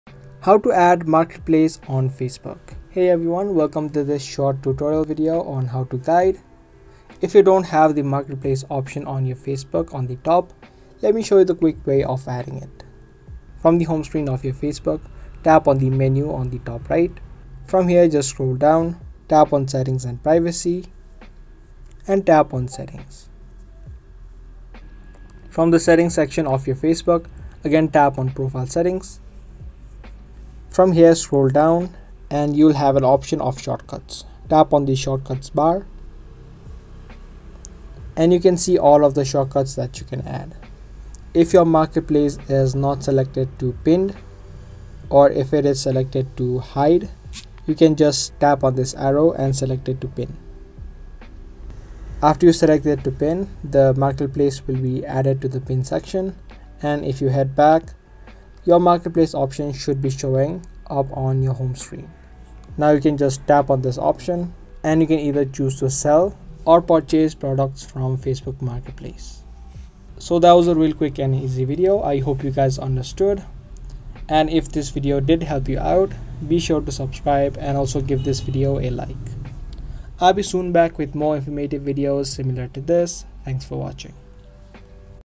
How To Add Marketplace To Facebook (2023 Update) | Fix Facebook Marketplace Option Not Showing
Learn how to add marketplace to facebook shortcut bar. Quick and easy tutorial on adding marketplace to your facebook profile

If you're looking to expand your Facebook experience and tap into the power of online shopping, consider adding Facebook Marketplace to your profile. In this tutorial, we'll show you how to add Marketplace to Facebook in just a few easy steps. Watch our visual steps on how to add marketplace to your Facebook profile is an easy way to discover new opportunities and connect with buyers and sellers in your area. Give it a try today!